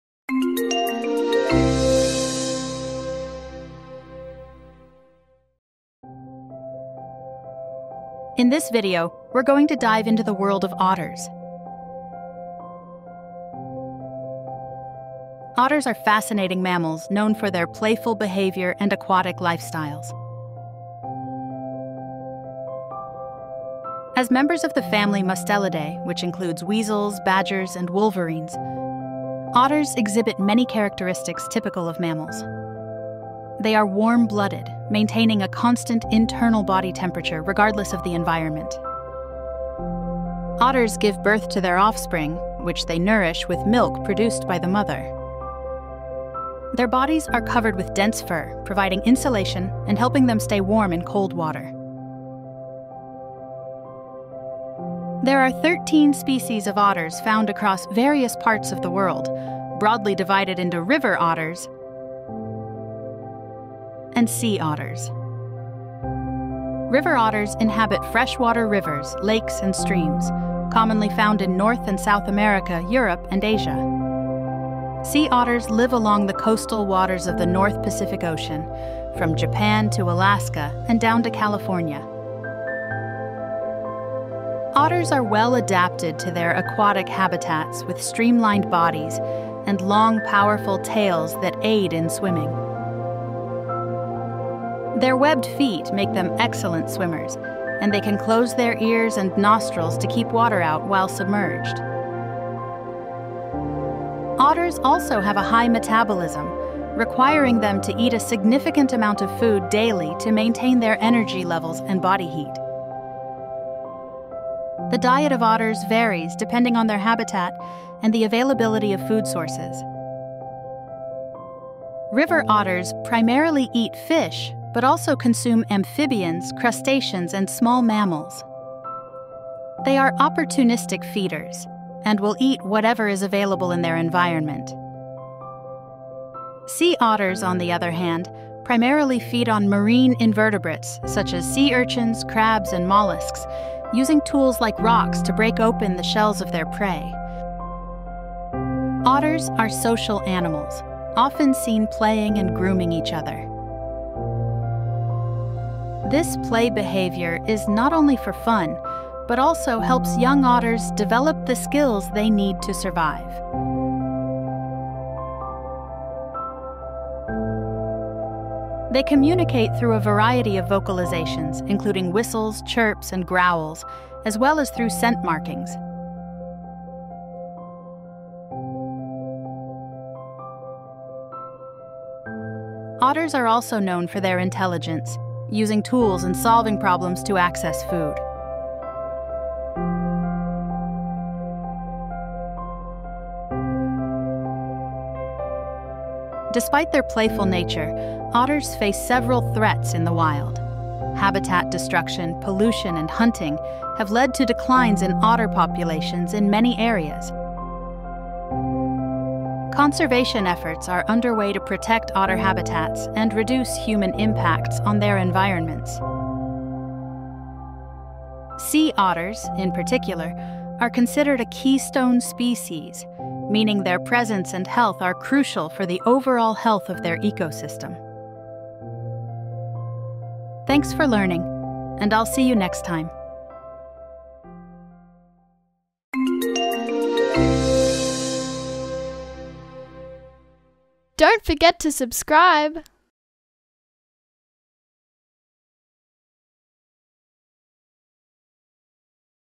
Otterly Adorble: The Life of Otters
otters
Otters are fascinating mammals known for their playful behavior and aquatic lifestyles. As members of the family Mustelidae, which includes weasels, badgers, and wolverines, otters exhibit many characteristics typical of mammals. They are warm-blooded, maintaining a constant internal body temperature regardless of the environment. Otters give birth to live young, which they nourish with milk produced by the mother. Their bodies are covered with dense fur, providing insulation and helping them stay warm in the cold water they typically live  .

There are thirteen species of otters found across various parts of the world, broadly divided into river otters and sea otters. River otters inhabit freshwater rivers, lakes, and streams, commonly found in North and South America, Europe, and Asia. Sea otters live along the coastal waters of the North Pacific Ocean, from Japan to Alaska and down to California.

Otters are well-adapted to their aquatic habitats with streamlined bodies and long, powerful tails that aid in swimming. Their webbed feet make them excellent swimmers, and they can close their ears and nostrils to keep water out while submerged. Otters also have a high metabolism, requiring them to eat a significant amount of food daily to maintain their energy levels and body heat.

The diet of otters varies depending on their habitat and the availability of food sources. River otters primarily eat fish but also consume amphibians, crustaceans, and small mammals. They are opportunistic feeders and will eat whatever is available in their environment. Sea otters, on the other hand, primarily feed on marine invertebrates such as sea urchins, crabs, and mollusks, using tools like rocks to break open the shells of their prey.

Otters are social animals, often seen playing and grooming each other. This play behavior is not only for fun but also helps young otters develop the skills they need to survive. They communicate through a variety of vocalizations, including whistles, chirps, and growls, as well as through scent markings. Otters are also known for their intelligence, using tools and solving problems to access food.

Despite their playful nature, otters face several threats in the wild. Habitat destruction, pollution, and hunting have led to declines in otter populations in many areas. Conservation efforts are underway to protect otter habitats and reduce human impacts on their environments. Sea otters, in particular, are considered a keystone species, meaning their presence and health are crucial for the overall health of their ecosystem. Efforts to protect sea otters have positive ripple effects on the entire marine environment.

Otters are a beloved and important part of the natural world, captivating people with their playful antics and vital ecological roles. As mammals, their warm-blooded nature, fur-covered bodies, and nurturing behavior towards their young highlight their adaptability and the importance of their conservation.